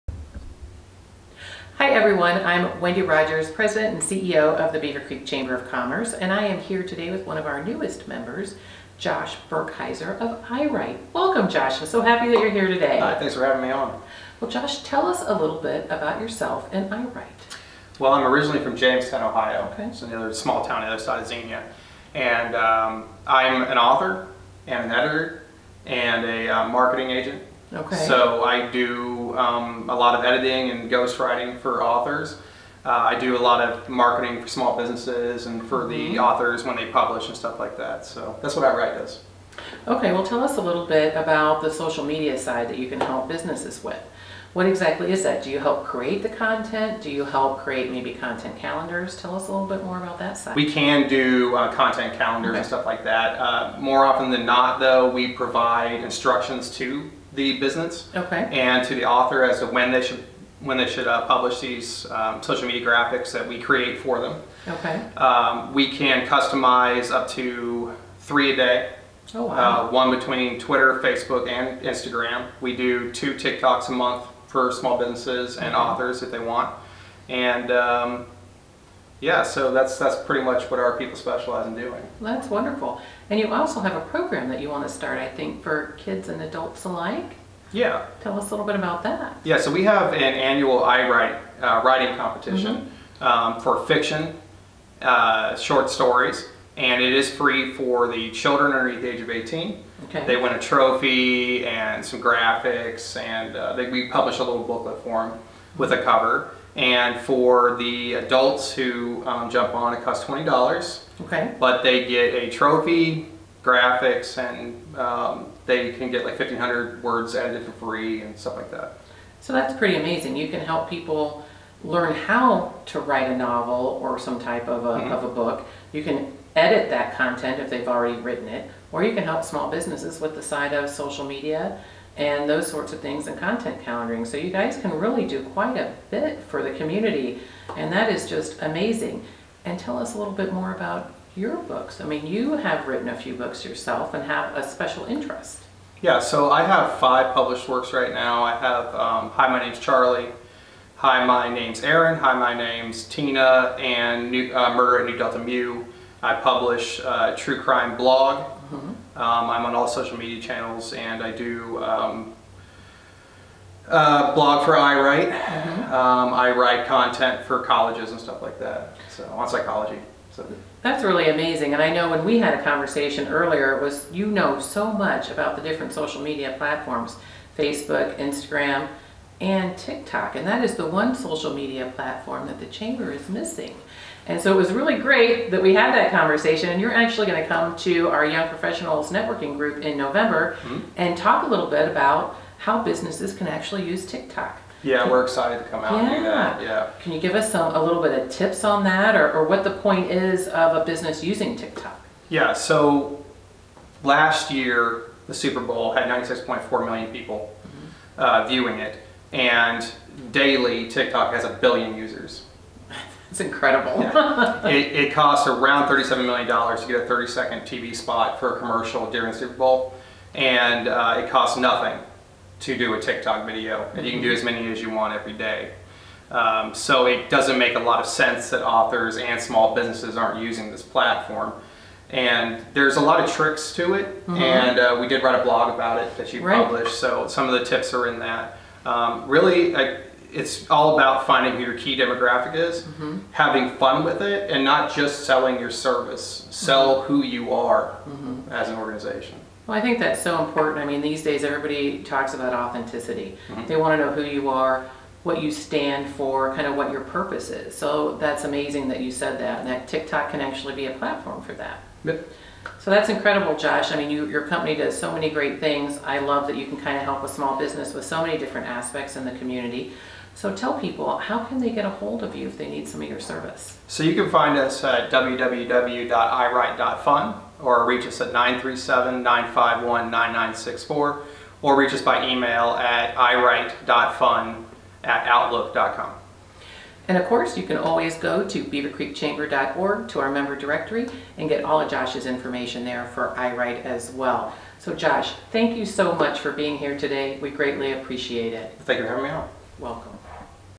iWrite LLC Member Interview - October 2022
Beavercreek Chamber of Commerce President & CEO Wendy Rodgers sat down with Joshua Burkheier, owner of iWrite LLC to learn about his editing, social media content, and ghostwriting services.

or search for him on our member directory